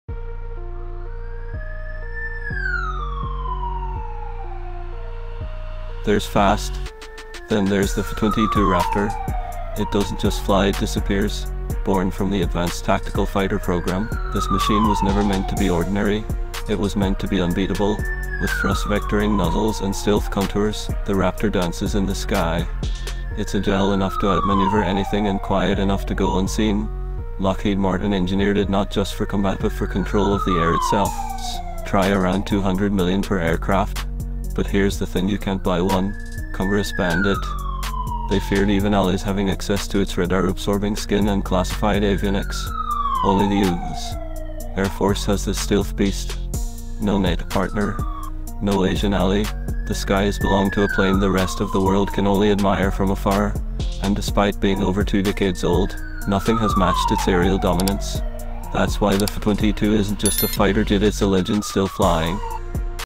United States Air Force USAF - F22 Raptor - Why the F 22 Was Too Good to Sell
The F-22 Raptor is the most powerful stealth fighter jet the world has ever seen—and no other country is allowed to own one. Built by Lockheed Martin and exclusively flown by the U.S. Air Force, the Raptor combines unmatched speed, stealth, agility, and advanced avionics. With a price tag of up to $200 million per jet, this 5th-generation warplane remains America’s untouchable guardian of the skies.
In this video, we break down the specs, the weapons systems, the reason behind the export ban, and what makes the F-22 a living legend in military aviation.
💥 Speed: Mach 2.25
🎯 Weapons: AIM-120, AIM-9, JDAM
🛡️ Stealth: Radar cross-section of a bird
💰 Cost: Up to $200 million
🚫 Export: Banned by U.S. Congress